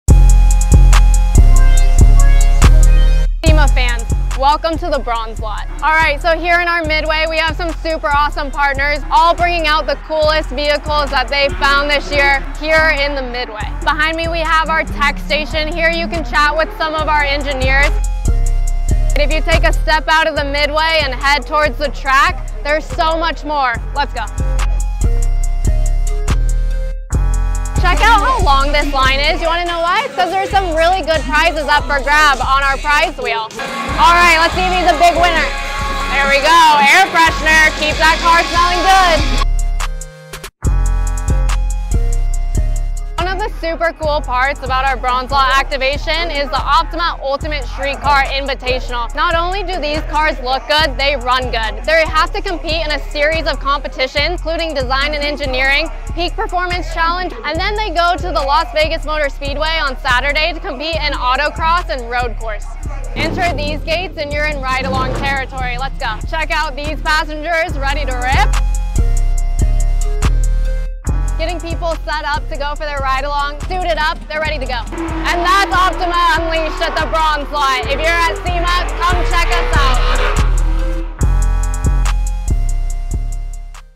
OPTIMA Unleashed at the 2024 SEMA Show
Welcome to the Bronze Lot, home of OPTIMAbatteries Unleashed action during SEMA week. The OPTIMA team is spinning tires and prize wheels, putting the ultimate street cars to the test, and hanging out with our top drivers all week long. Don't miss it!